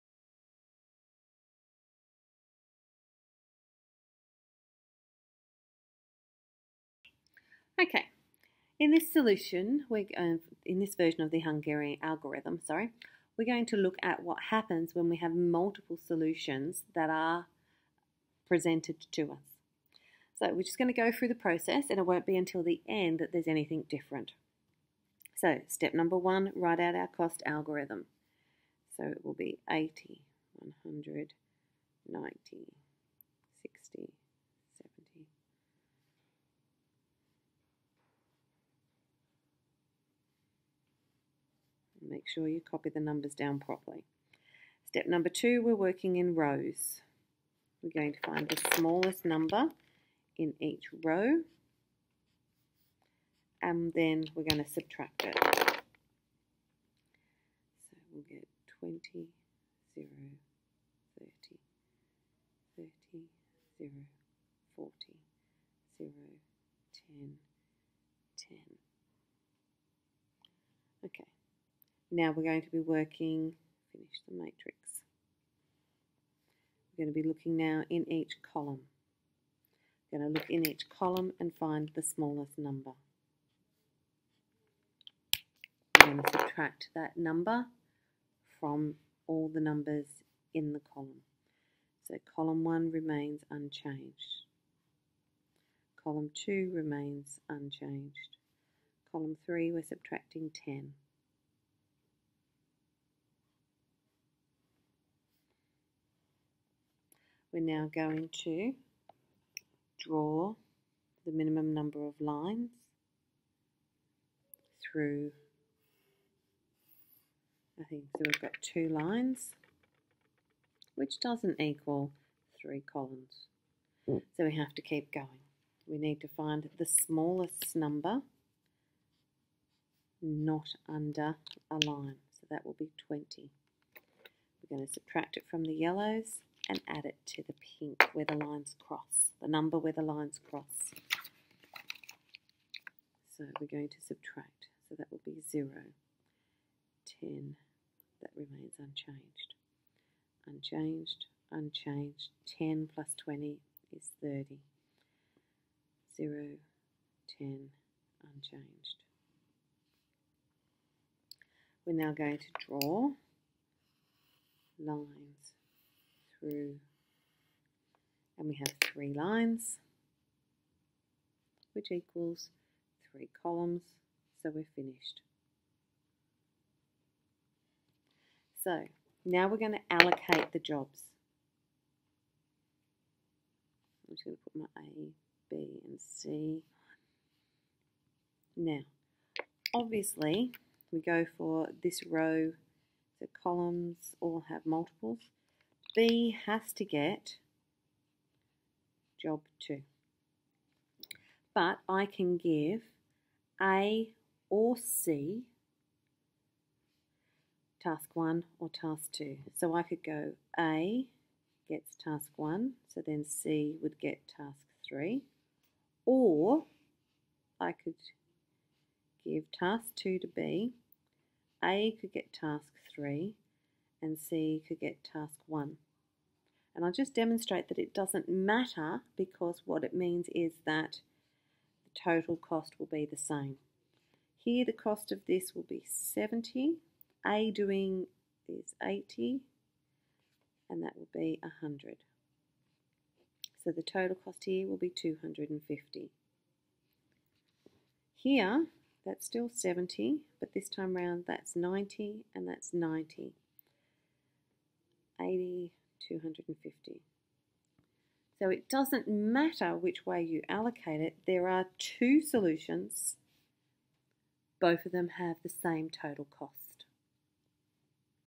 Hungarian Algorithm - What to do with multiple solutions
How to use the Hungarian Algorithm to solve assignment problems that can have multiple solutions